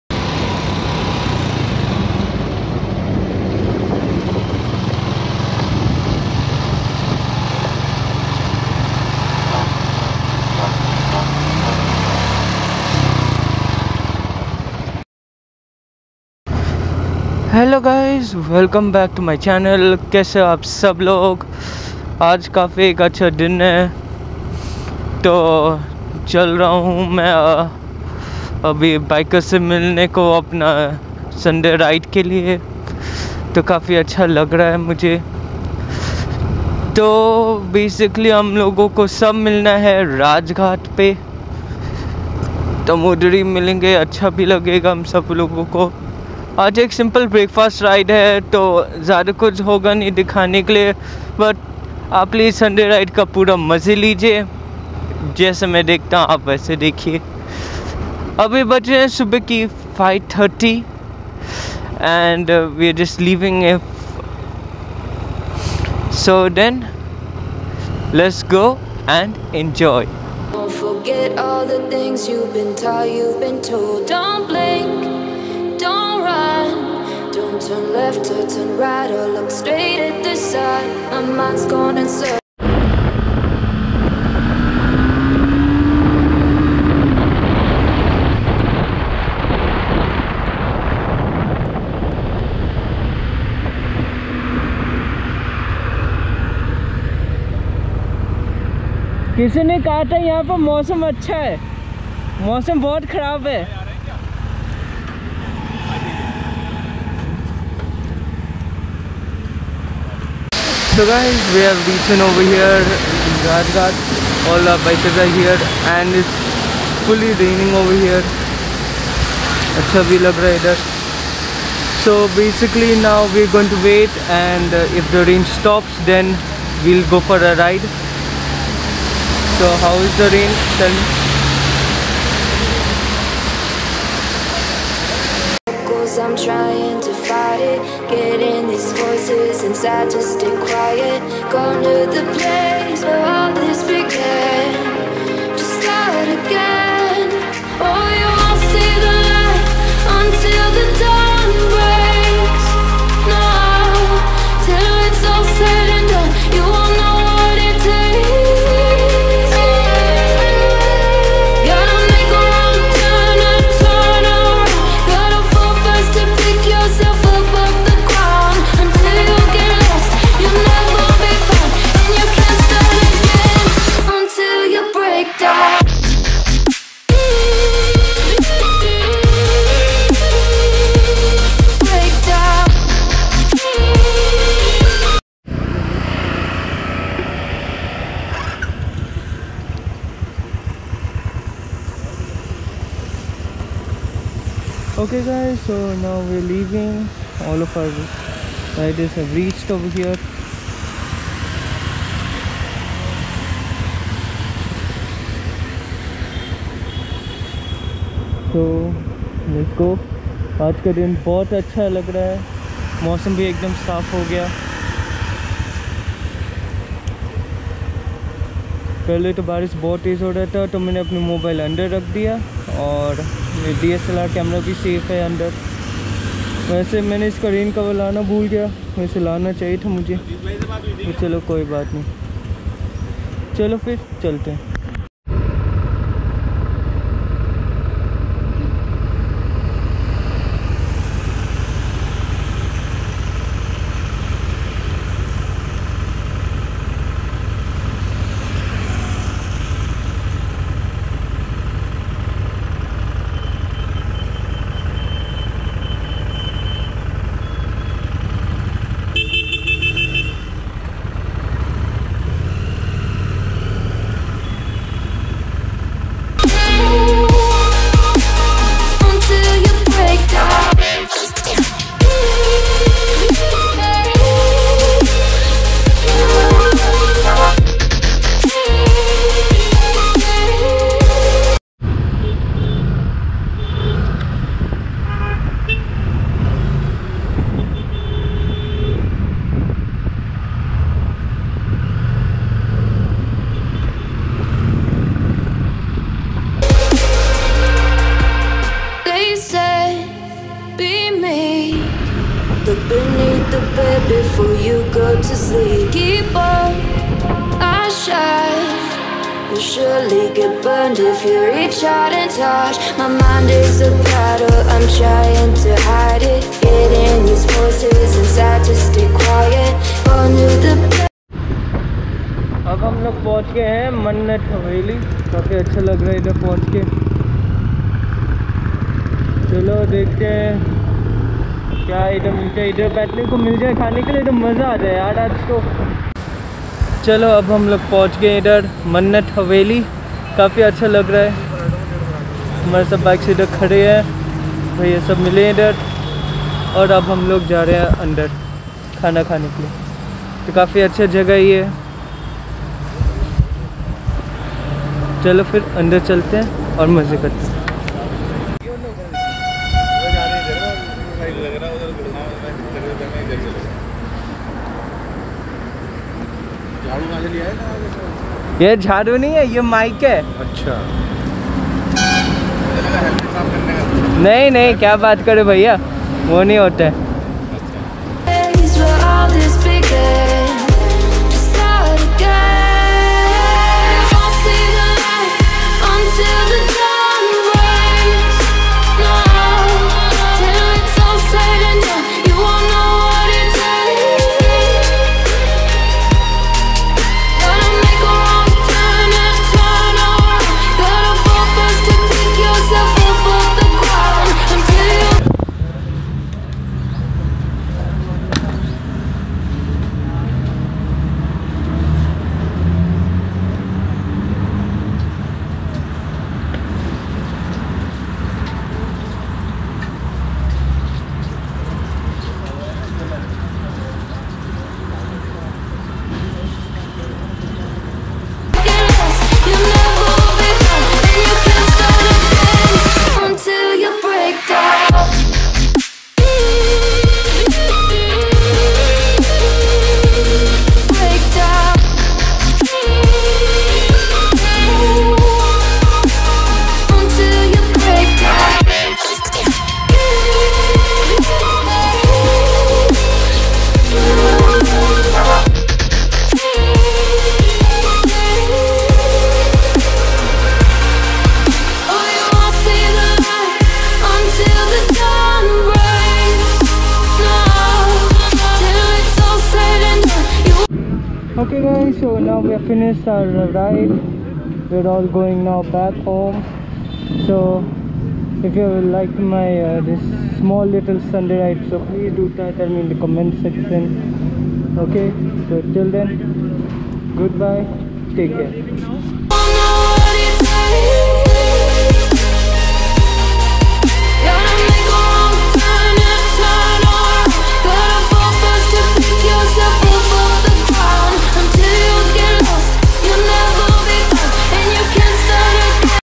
Sunday ride and a beautifully day
i am going on my sunday ride and enjoying